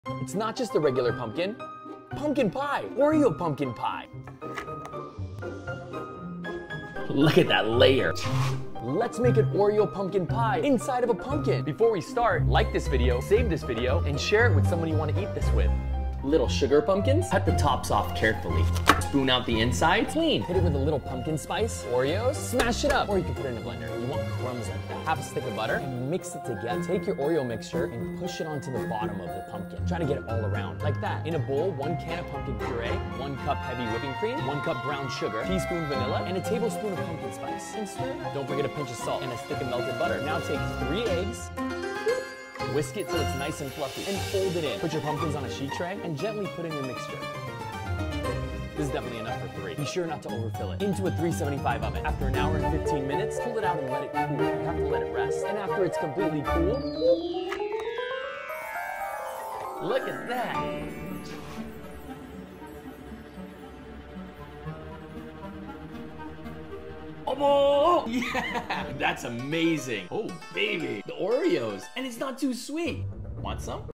Oreo Pumpkin Pie Inside a Pumpkin … Abowwww
Oreo Pumpkin pie inside of a pumpkin! I came up with this fun idea and it worked out awesome! It’s a small pumpkin with layers of Oreo crust and pumpkin pie and you can eat the whole thing together all at once just don’t eat the skin haha. This Holiday season can be much more fun when you bring a pie in a pumpkin to your next party or dinner.
3 small pumpkins, inside removed
2 tubes of Oreos, crushed
1/2 stick butter, cold
1 stick butter, melted
1 can pumpkin puree
1 cup heavy cream
1 cup brown sugar
1 tbsp vanilla extract
1 Tb pumpkin spice
3 eggs
- press together crushed Oreos and 1/2 stick butter to form paste
- spread paste all over inside pumpkin
- Mix together all other ingredients and divide into pumpkins *besure not to overfill, you need room for it to rise
- cook uncovered in preheated 375 oven for 1 hour and 15 min or until the custard stiffens up. Let it cool completely until enjoying